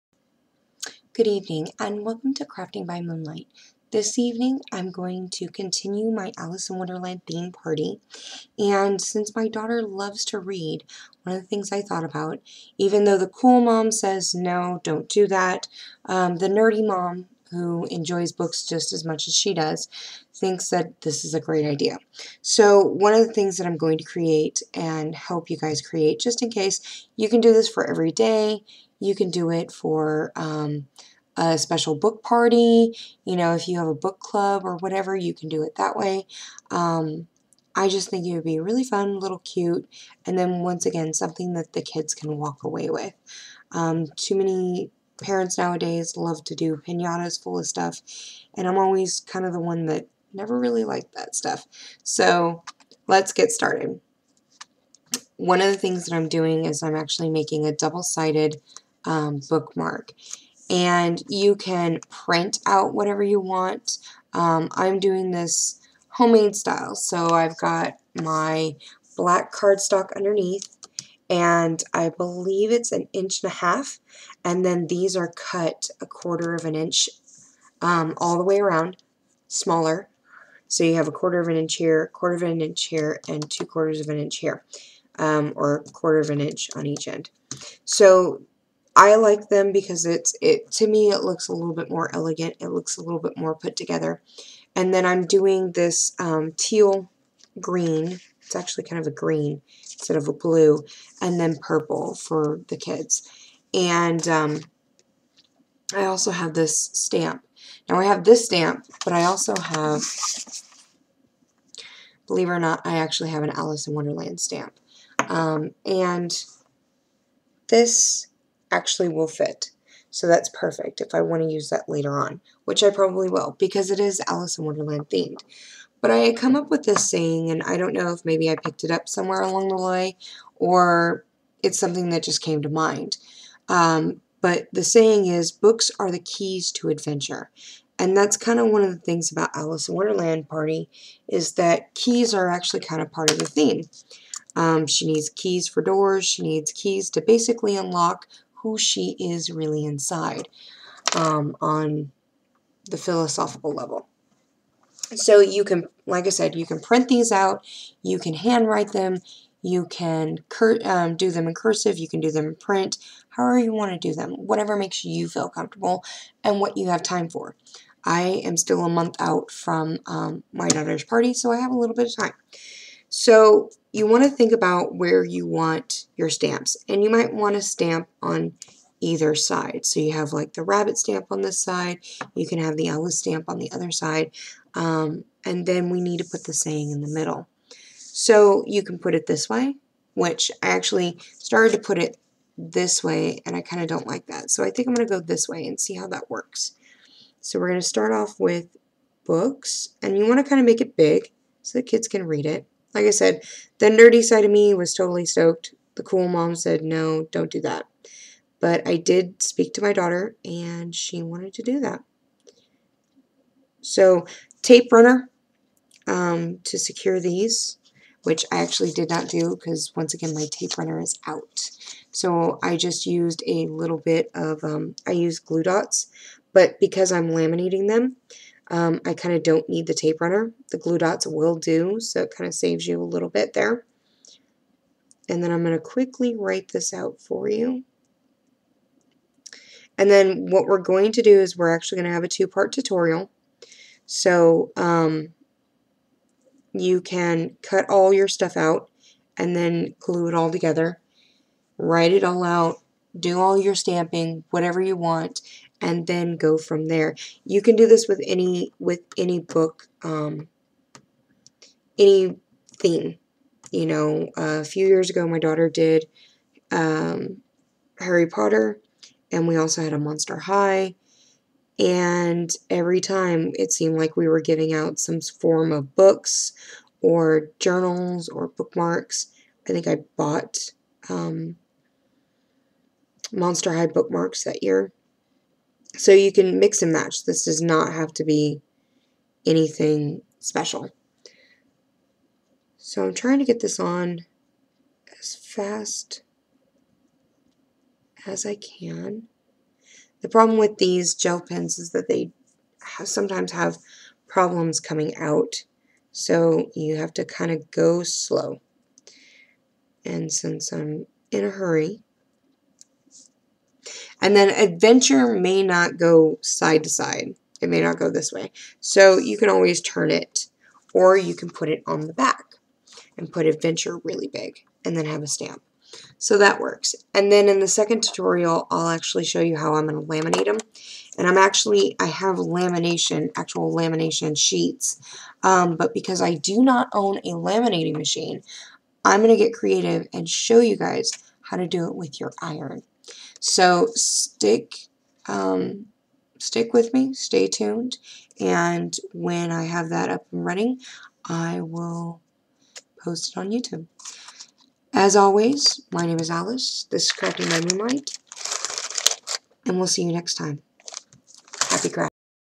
Alice Book Marks
these book marks make good party favors. You can customize how ever you want. You can make them for your book club.